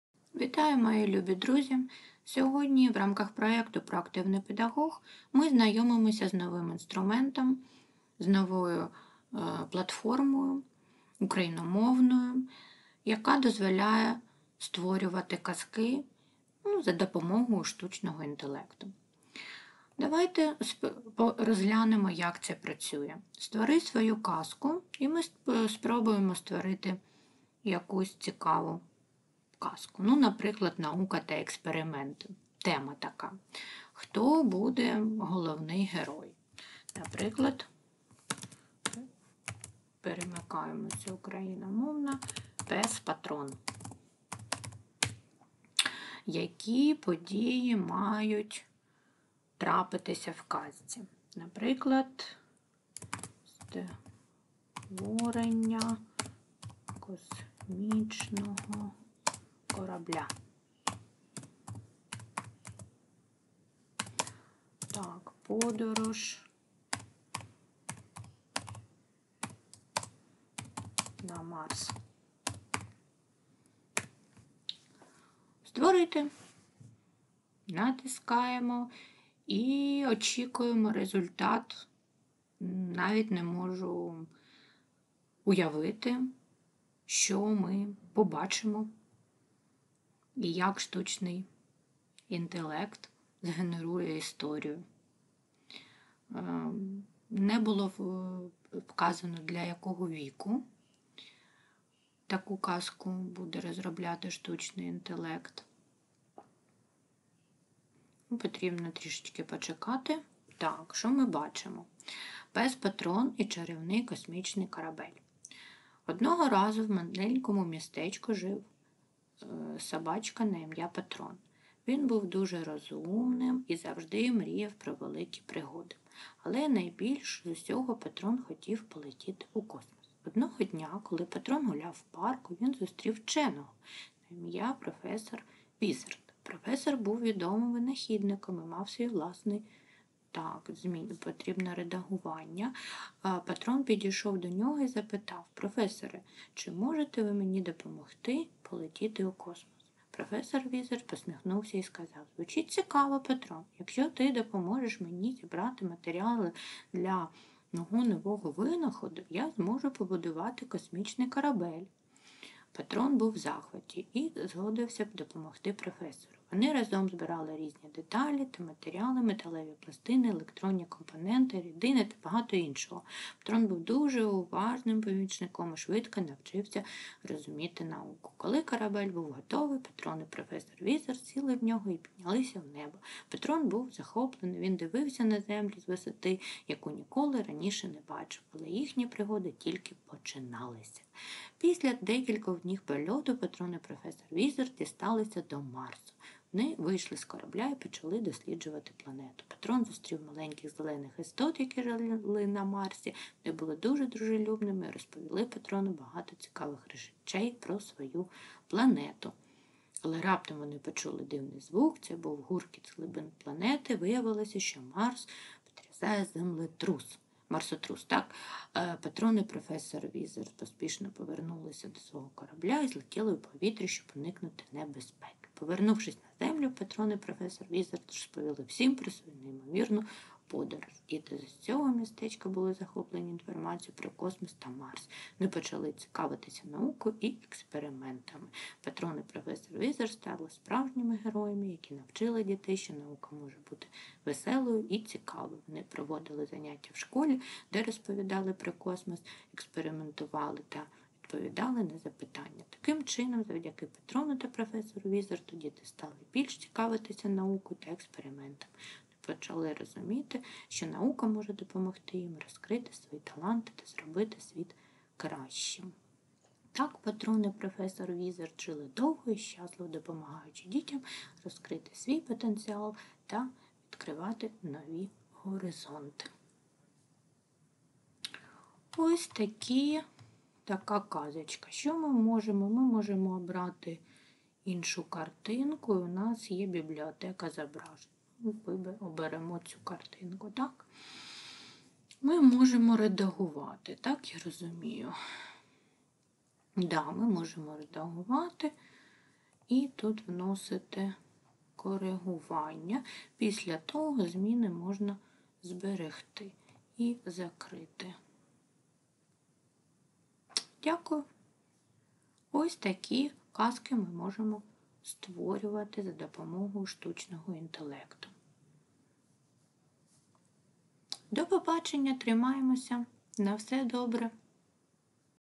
"Kazka.fun": безкоштовний генератор казок (штучний інтелект)
"Kazka.fun" — це не просто проєкт, це справжня казкова Одисея для маленьких відкривачів таємниць. Дозвольте розповісти вам про цей неймовірний світ, де об'єднались казки, технології та добрі наміри.

5 генерацій казок на день

Натисни кнопку створи казку.

Бережіть себе і родину! Все буде Україна!